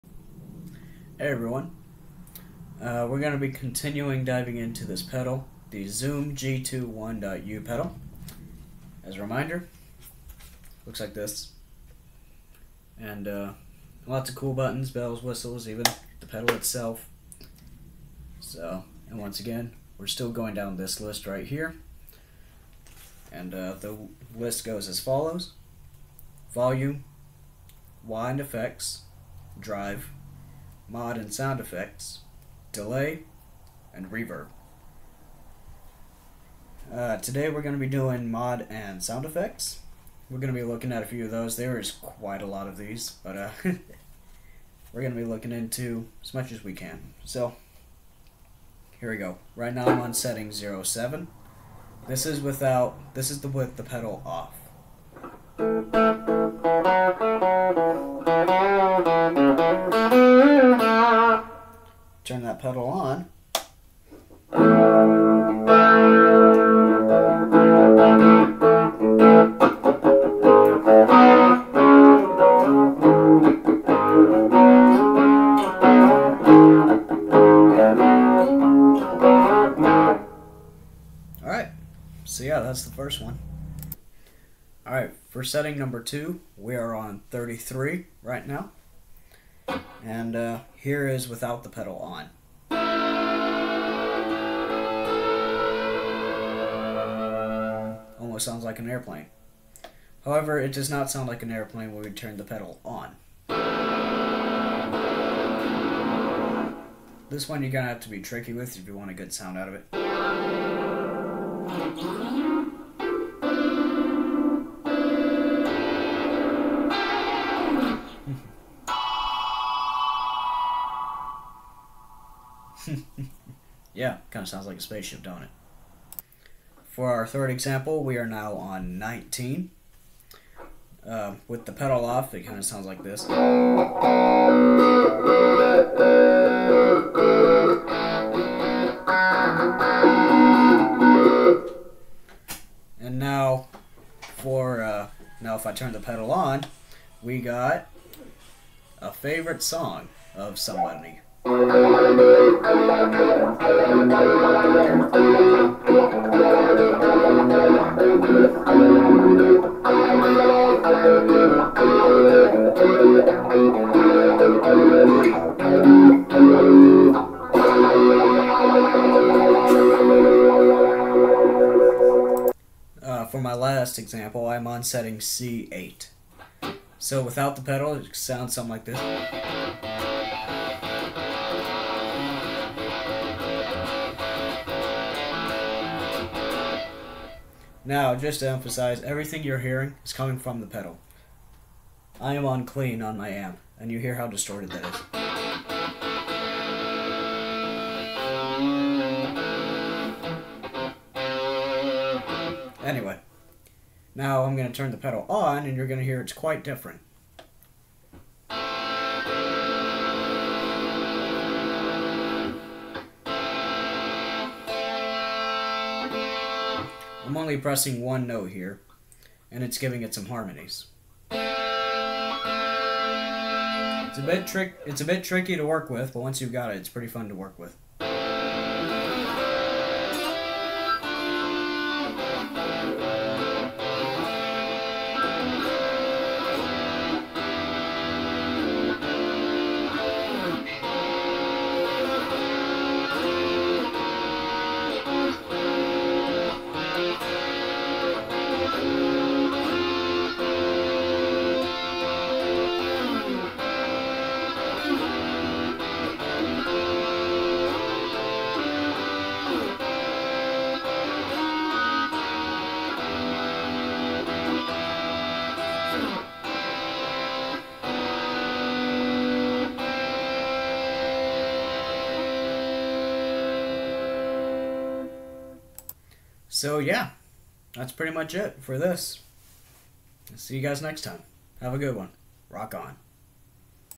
Zoom G2.1u PEDAL: "MOD/SFX"
Yassas!
There are many Mods and SFX you can do with this pedal. All you gotta do is try it out and experiment! Have fun!

*Gear:*
Samick stratocaster
Crate amp
Zoom G2.1u pedal